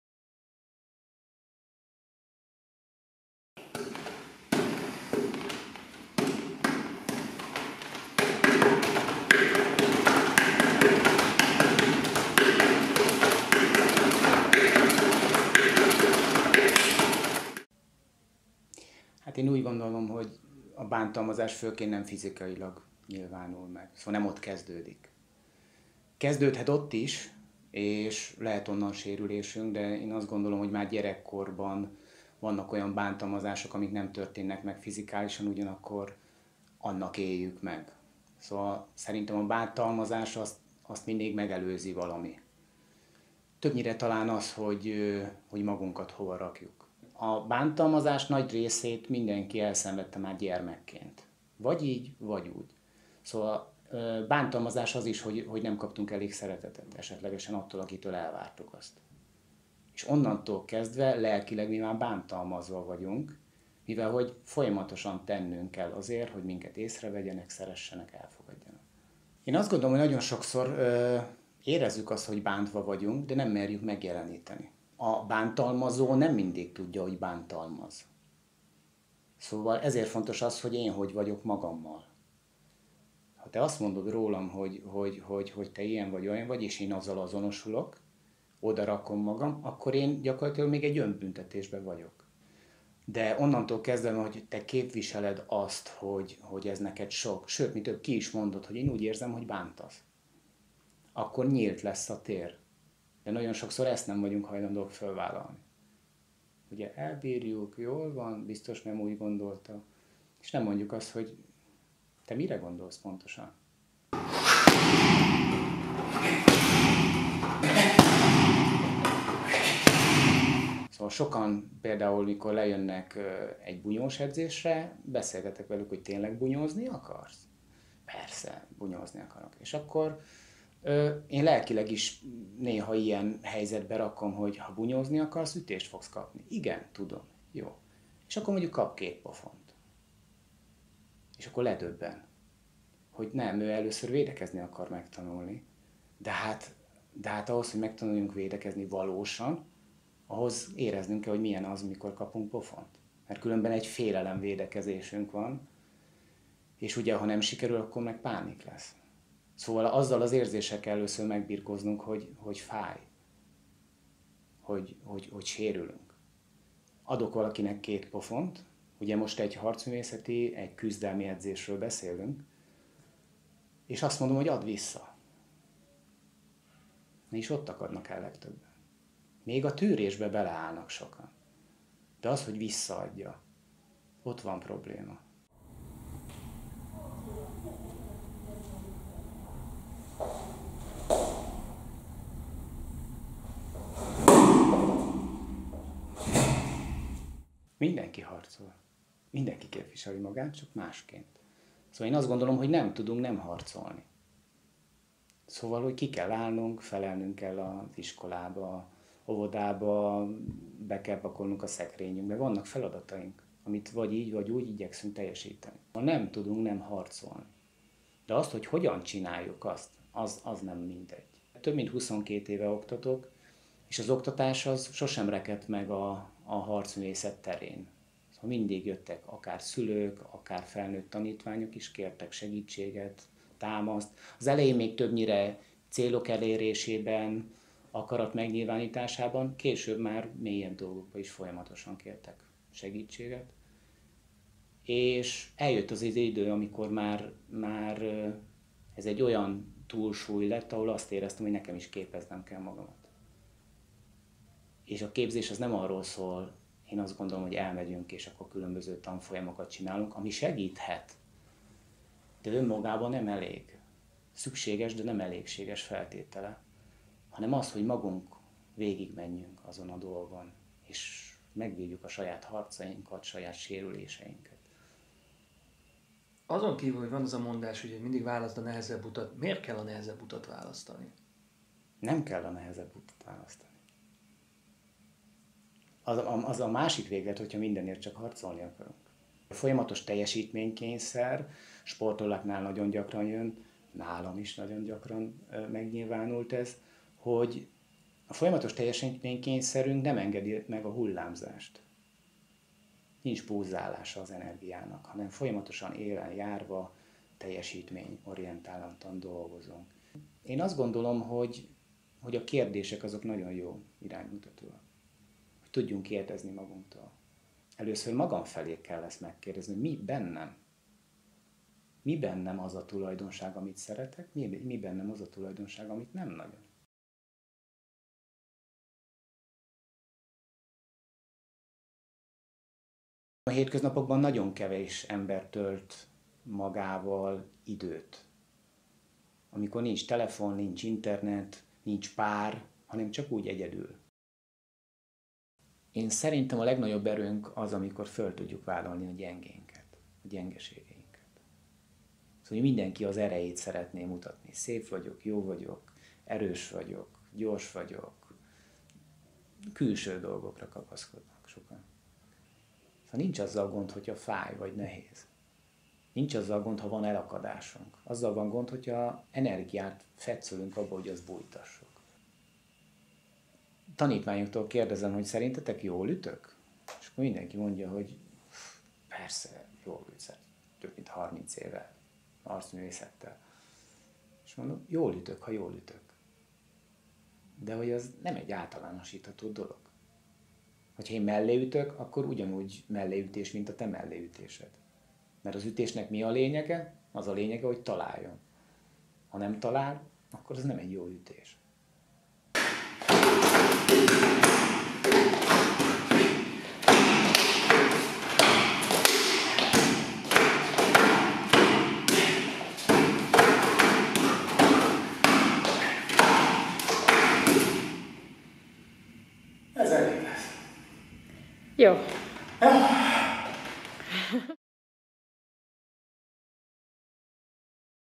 Beszélgetés Ambruzs Szabolcs négyszeres kung-fu világbajnokkal - MyMirror Magazin
Beszélgetés Ambruzs Szabolcs négyszeres kung-fu világbajnokkal, coach-tanácsadóval küzdelemről és elengedésről, erőről és gyengeségről, fizikai és lelki bántalmazásról - környezetünknek történő megfeleléseinkről és a saját lelkünkben vívott harcainkról.
Az interjú közben meg is vendégelt minket egy hagyományos kínai tea-szertartás keretében - köszönjük a lelki és fizikai feltöltődést!